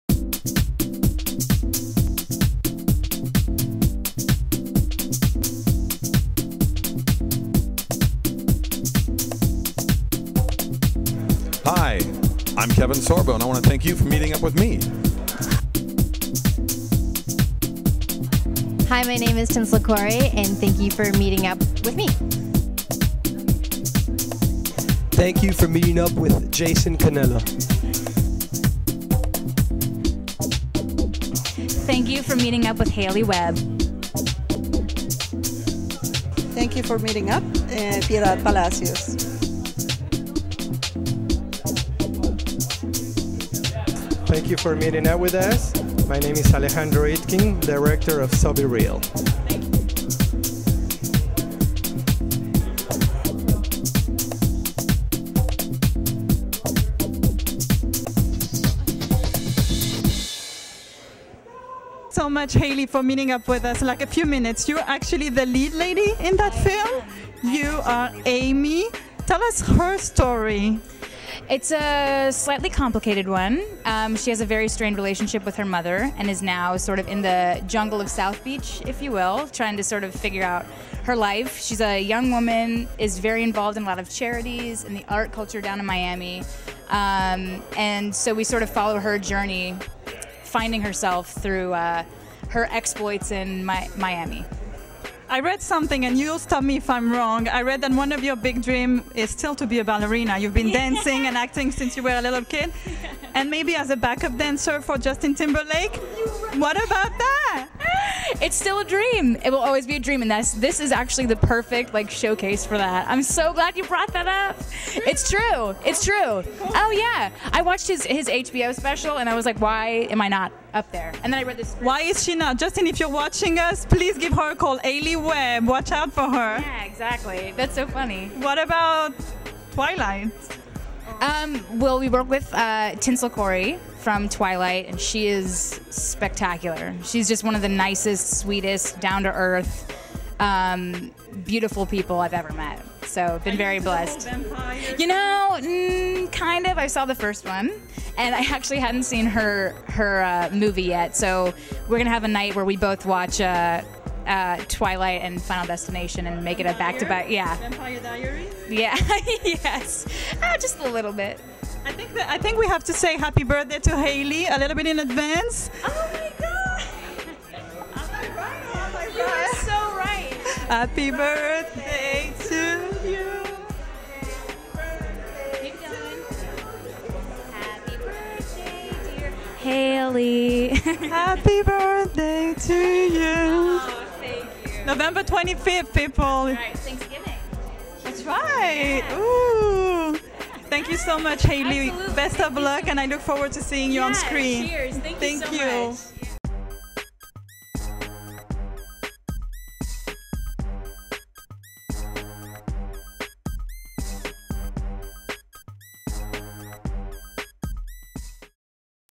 Haley Webb Interview.mov
Starring
Host- Yael Nemni
Guest Star
Haley Webb
Special Thanks to
Matt Hein "Eastside PR"
Avant Gallery
Alejandro Itkin - Director of "SoBe Real"
Piedad Palacio- Producer of "SoBe Real"
Crew
Videographer- De'ante Morton
Director/ Exec Producer- Beverley Harris-Alvarez

MUW - Meeting Up With is in Assoication with -- A Higher Calling Production Inc.
Meetingupwith.com
Ahcproductions.com